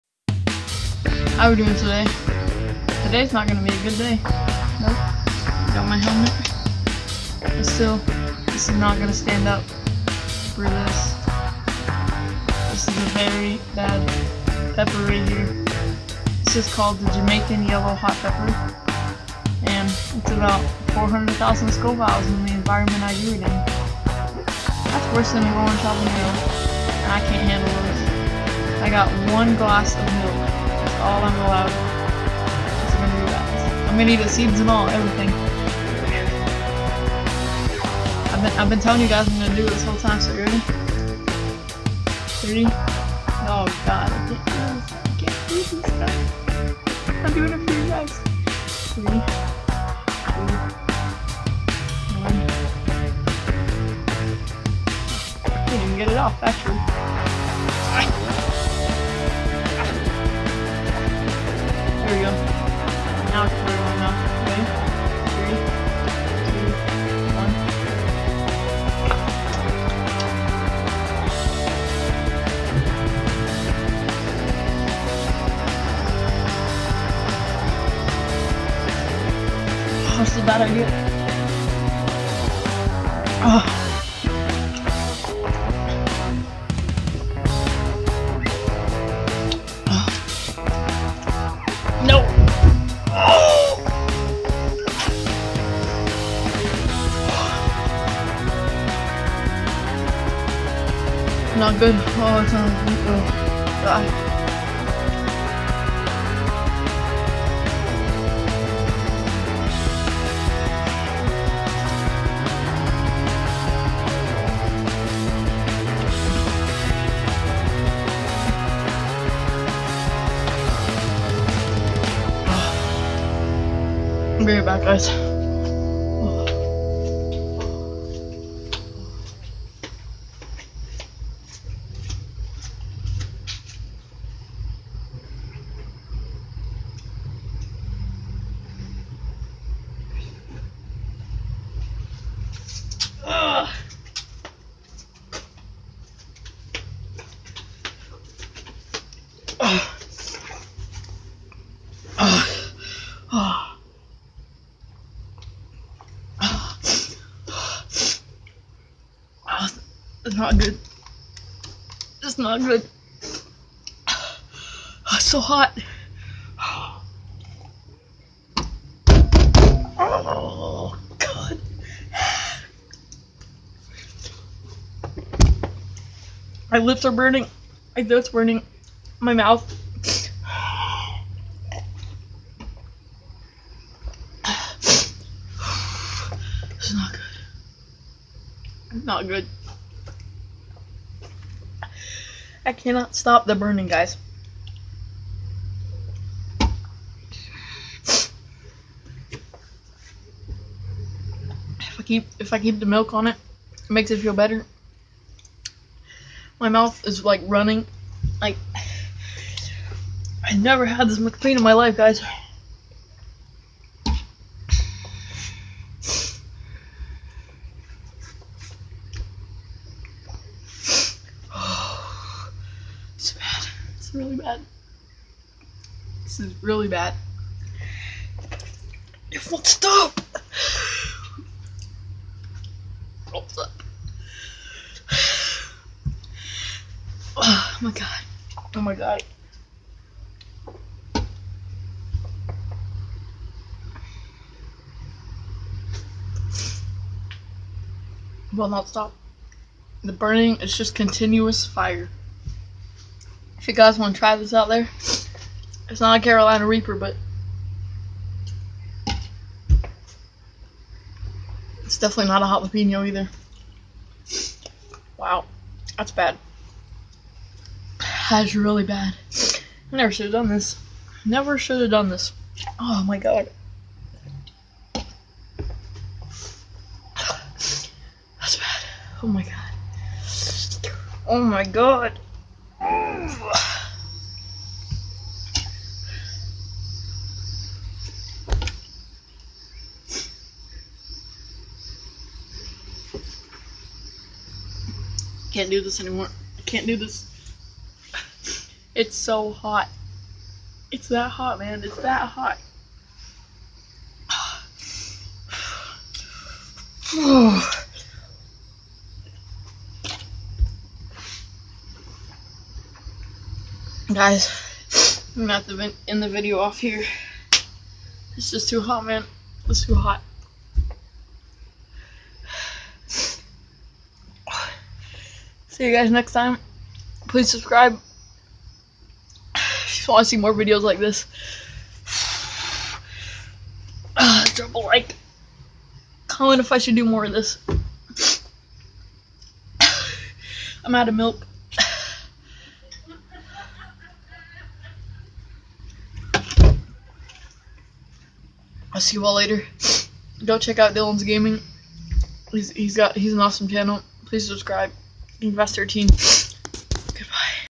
HOT Pepper Challenge- The Jamacian Pepper Bites Back
Hello guys today i am doing a very Painful thing. THIS IS OVER 400,000 SCOLVILLES OF HEAT! Try this at home but it is very painful!!!